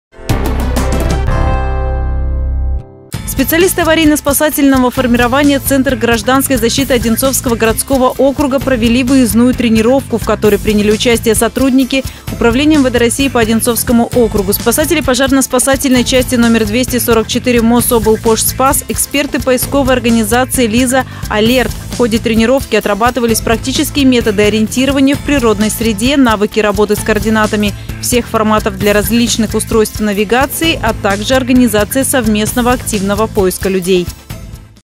Отработка взаимодействия
Специалисты аварийно-спасательного формирования  «Центр гражданской защиты Одинцовского городского округа» провели выездную тренировку, в которой приняли участие сотрудники УМВД России по Одинцовскому городскому округу, спасатели пожарноспасательной части №244 «Мособлпожспас», эксперты поисковой организации «Лиза Алерт».